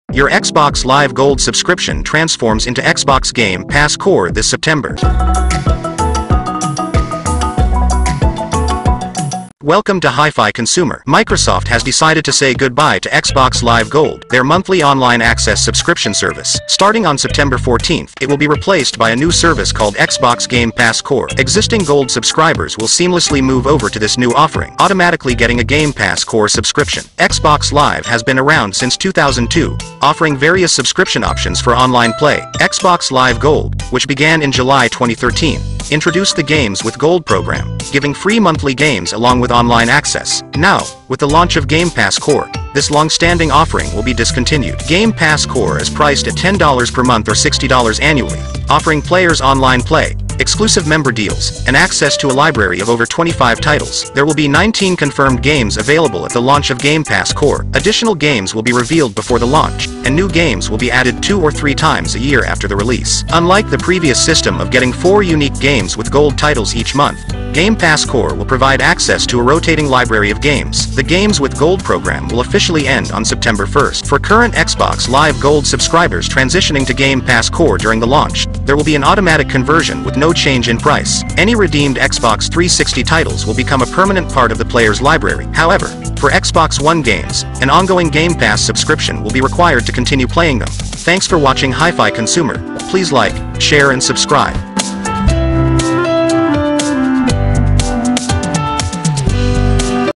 Subscription Will Automatically Change From Xbox Live Gold To Xbox Game Pass Core On September 14th
Video Chapters:
0:11 Good Bye To Xbox Live Gold
0:19 Xbox Game Pass Core Will Surprise You
0:46 Subscription Charges of Xbox Game Pass Core